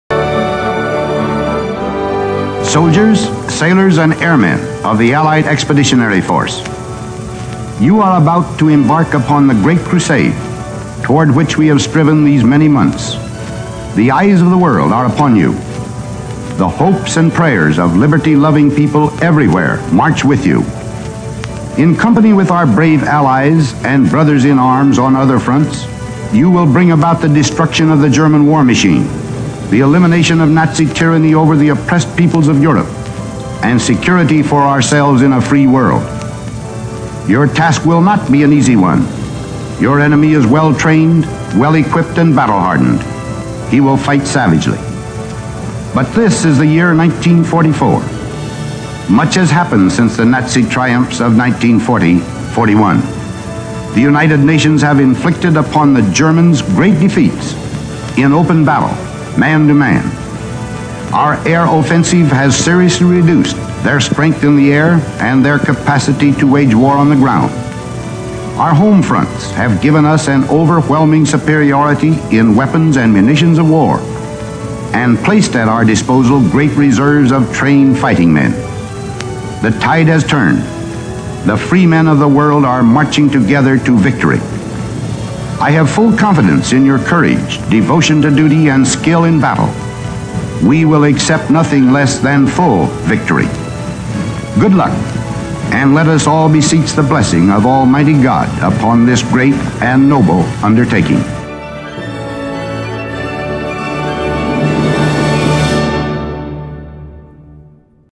Eisenhower's Inspirational D-Day Invasion Speech
Honoring the military on the 75 Anniversary of the Invasion of Normandy. 75 years and one day ago, Gen. Dwight D. Eisenhower delivered this inspiring speech the day before the invasion of Normandy. Listen to the actual speech, watch real footage of the invasion, see ground battles, bombings from the skies and grateful faces of those freed from Nazi tyranny. This historical invasion against the Germans on the shores of France took Hitler and his forces by surprise leading to the fall of Germany. Never forget the great sacrifices these historic WWII forces made to keep the world safe and most importantly free.